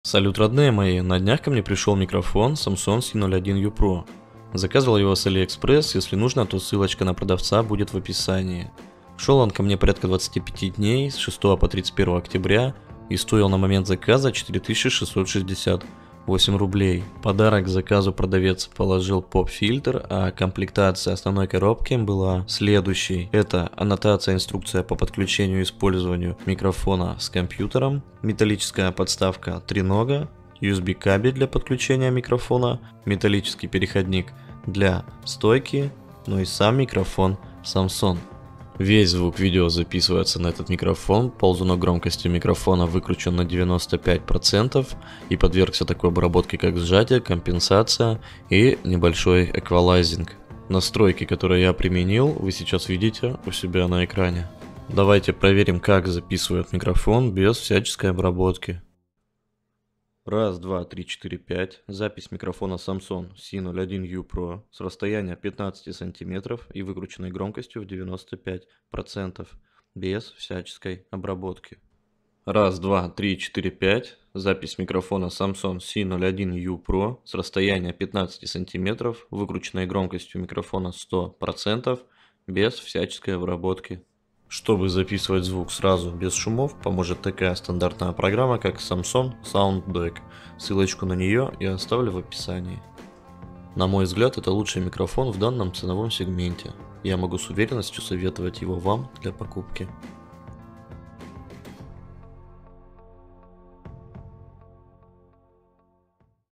Микрофон Samson c01uPRO с aliexpress
Салют, родные мои, вот и пришел ко мне новый микрофон, давайте затестим!

Ссылочка на магазин

Микрофон Samson c01uPRO с aliexpress I обработка + тест на windows 10 I Настройка 2020